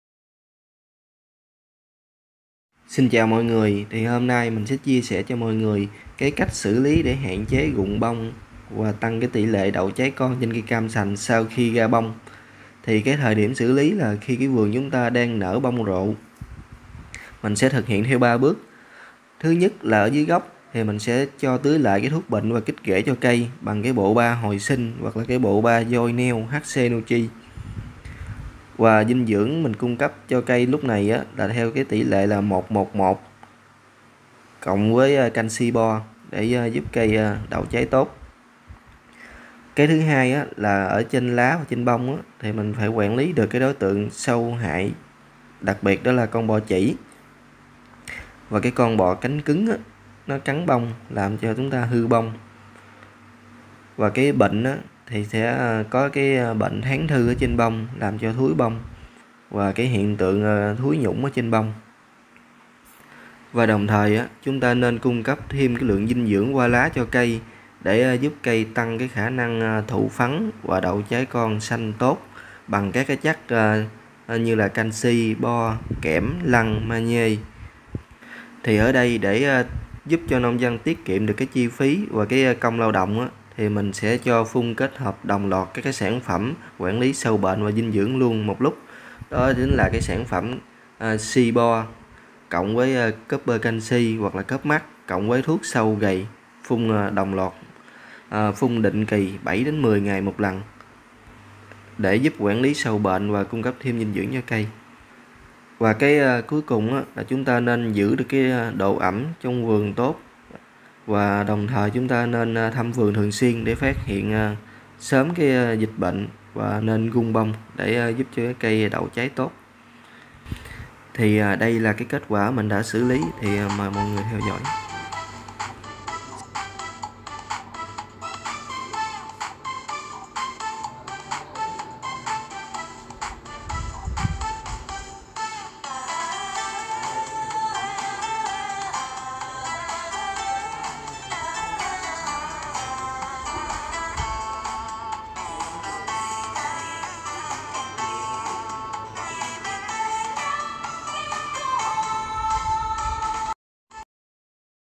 HẠN CHẾ RỤNG BÔNG & TRÁI NON TRÊN CAM SÀNH
Video chỉ mang tính chất tham khảo, chia sẽ 1 số kinh nghiệm thực tế cho bà con nông dân, phần nào hạn chế được vấn đề rụng bông, trái non trên cam sành.